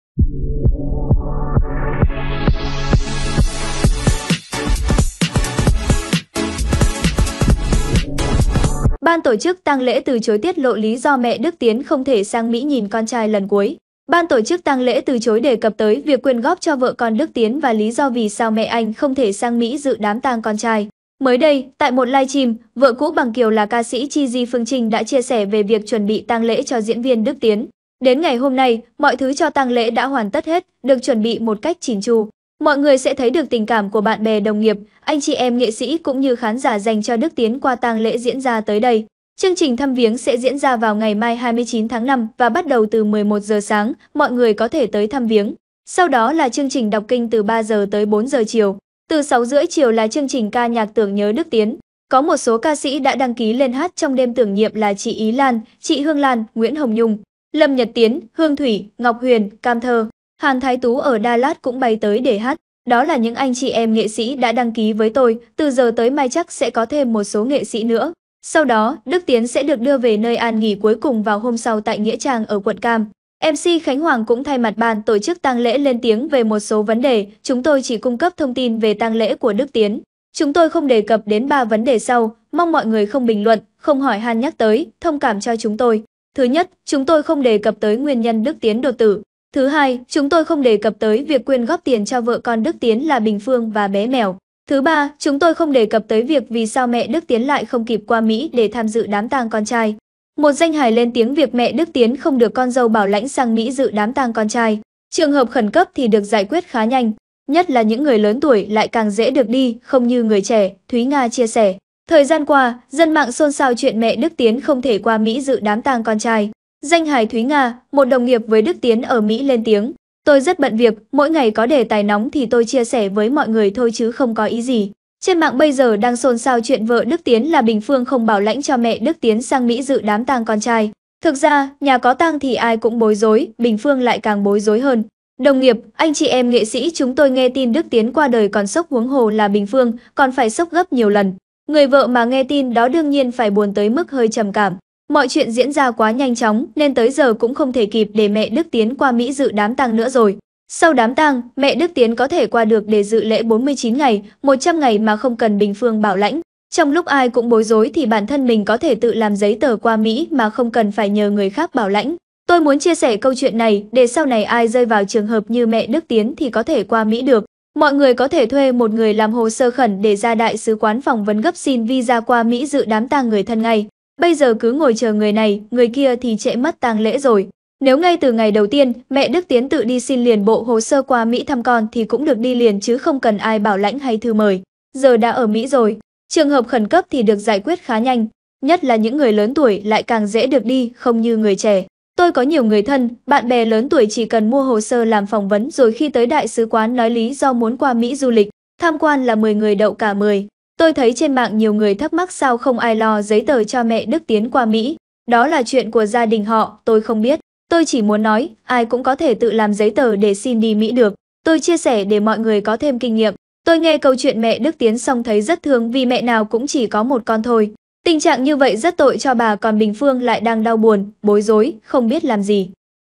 Vì Sao Ban Tổ chức Tang Lễ Cho Đức Tiến Lại Từ chối Tiết Lộ Chuyện Không Ký Giấy Cho Mẹ Anh Qua Mỹ
ductien đứctiến giaitri Sao Ban Tổ chức Tang Lễ Cho Đức Tiến Lại Từ chối Tiết Lộ Chuyện Không Ký Giấy Cho Mẹ Anh Qua Mỹ